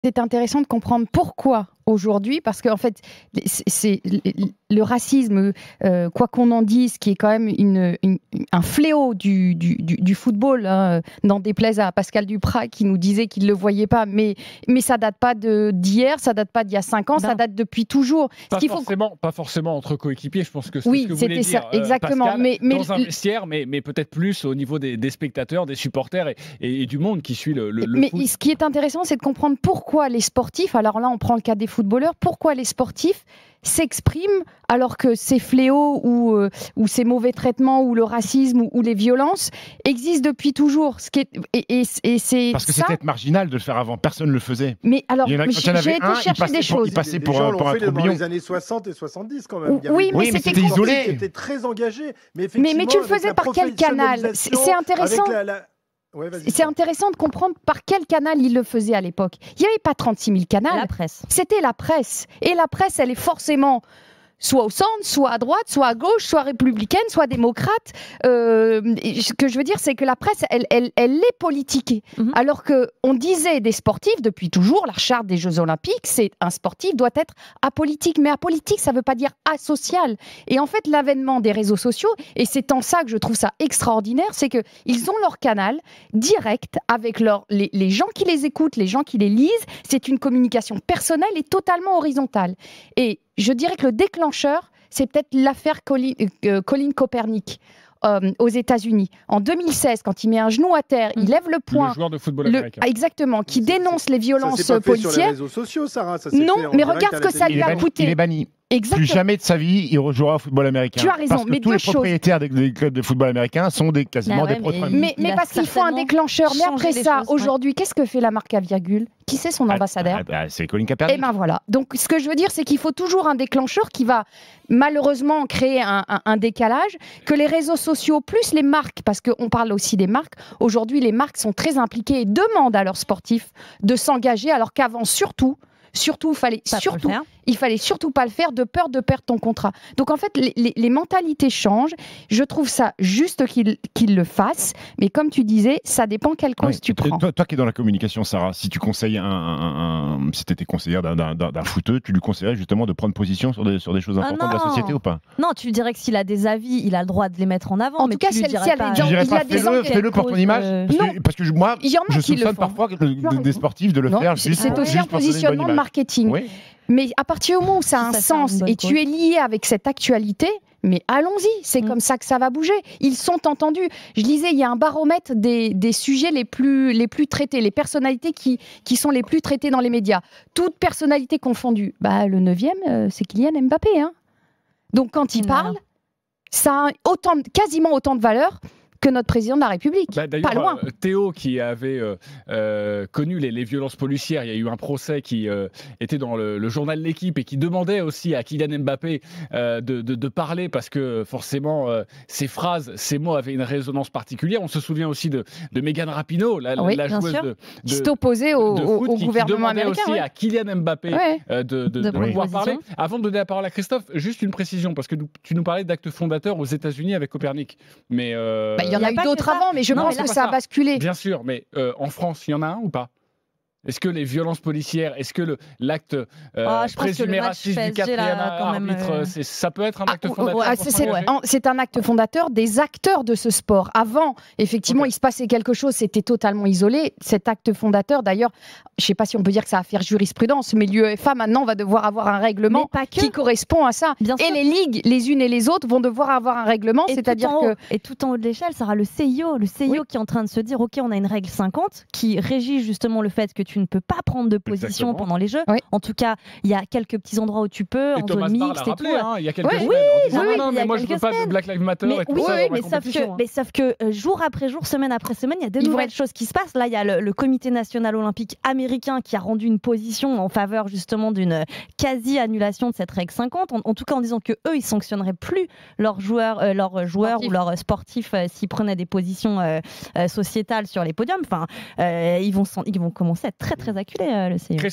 Griezmann, Mbappé... Pitkowski analyse pourquoi les sportifs se mobilisent
▶️ Toutes les vidéos sport sont à retrouver sur l’appli RMC Sport News